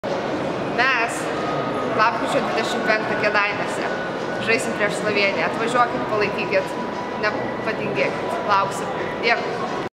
Marina Solopova
Lietuvos moterų krepšinio rinktinės žaidėja Marina Solopova kviečia į rungtynes su Slovėnija. Kėdainiuose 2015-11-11.